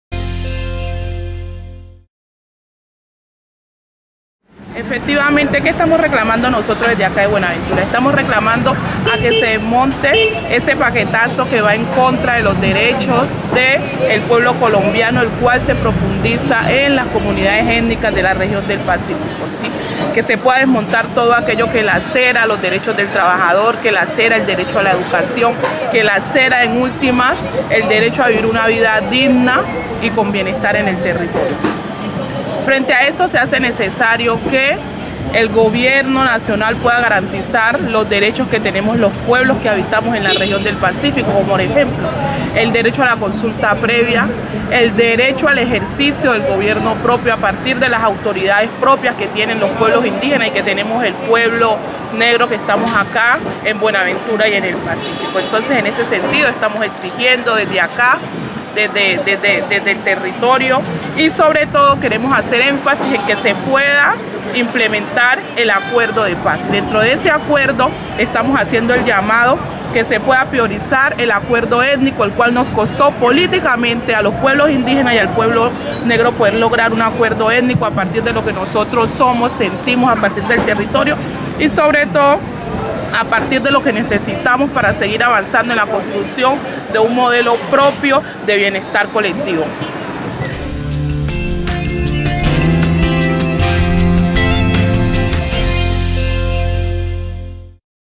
Urge cumplir con el capítulo étnico del acuerdo de paz
El Acuerdo étnico hace parte del Acuerdo de paz y busca avanzar en la construcción de un modelo propio de bienestar colectivo. Por la comisionada interétnica Mary Cruz Rentería Mina.